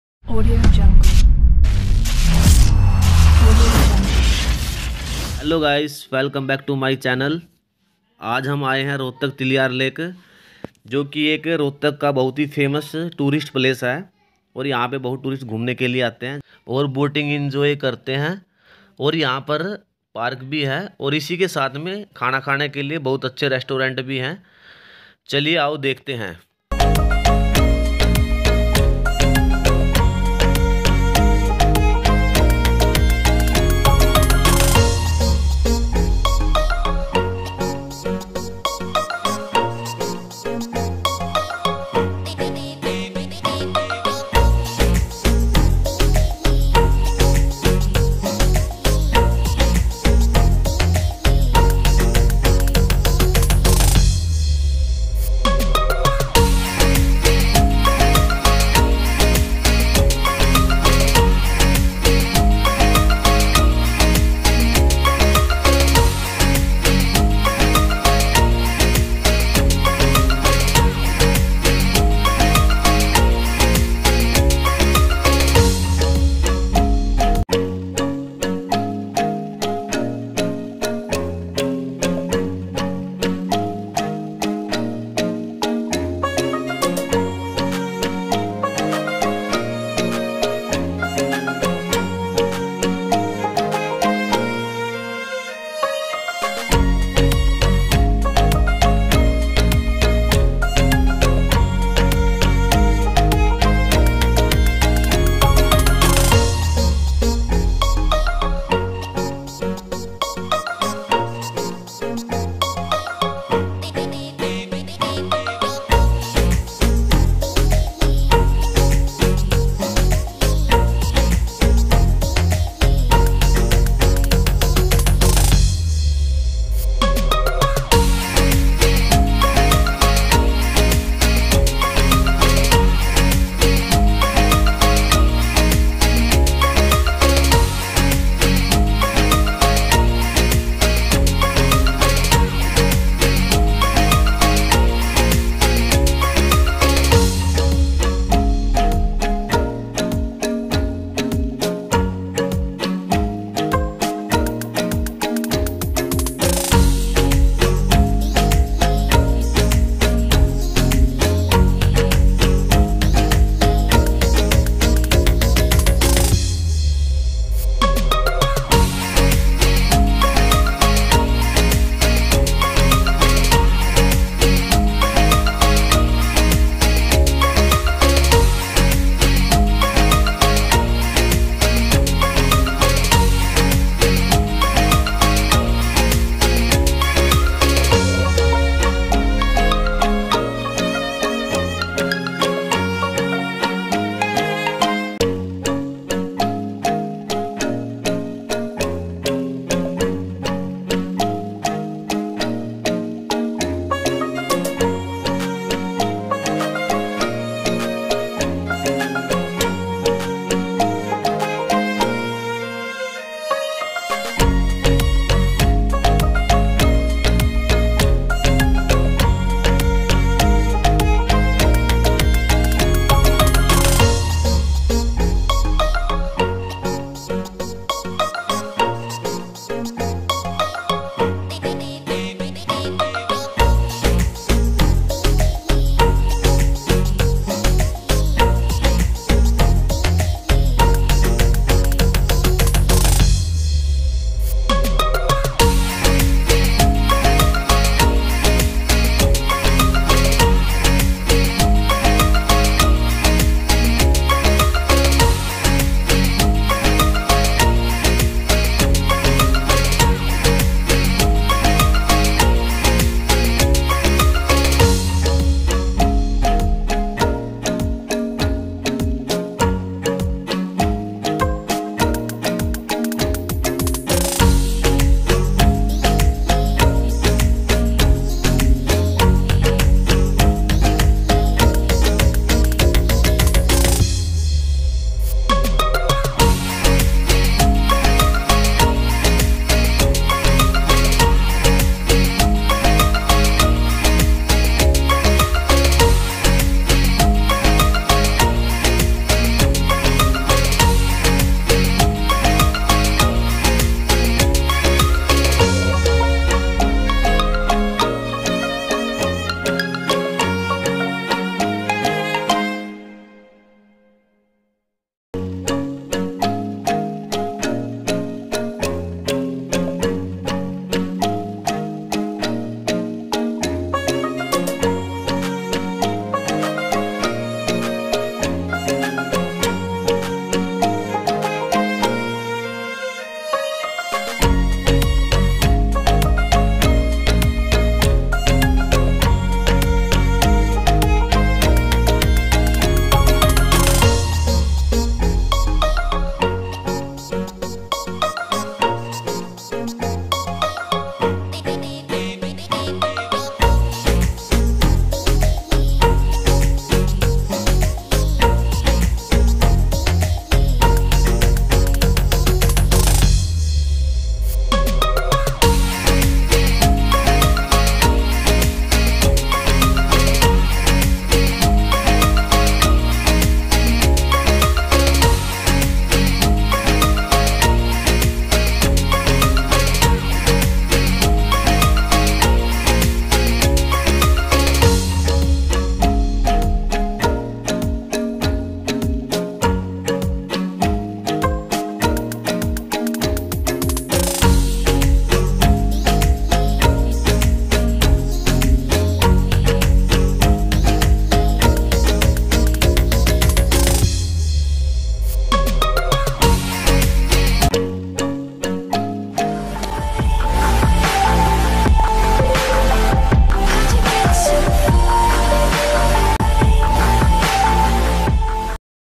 Rohtak Tilyar lake🌊||Shaket Verma vlogs
|| Tilyar lake Rohtak Nice tourist place for picnic,boating and zoo also nearby .